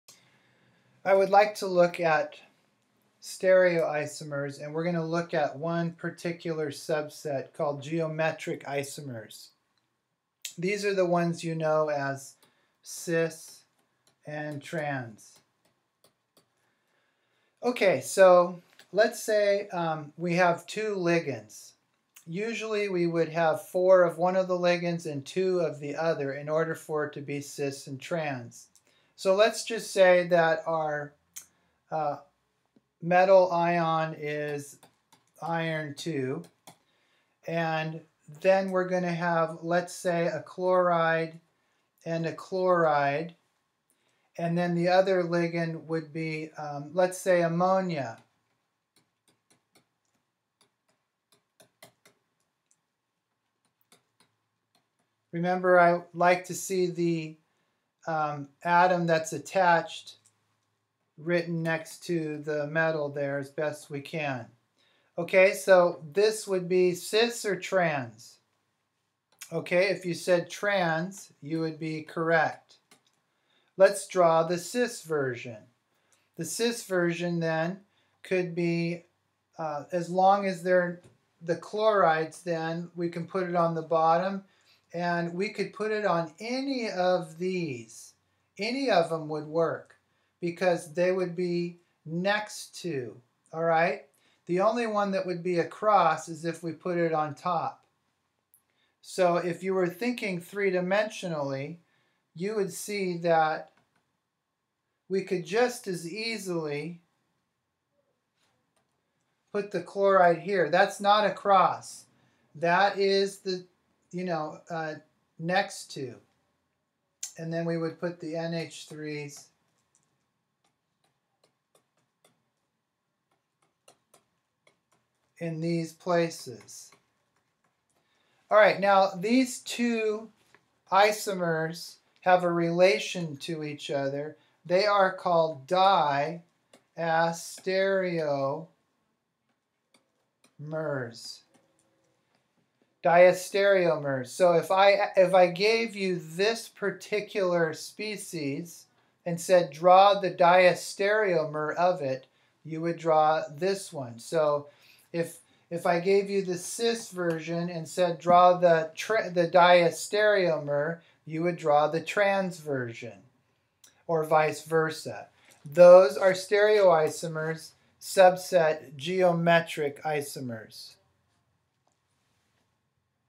Geometric isomers of complex diastereomers
How to draw geometric (cis/trans) isomers of a complex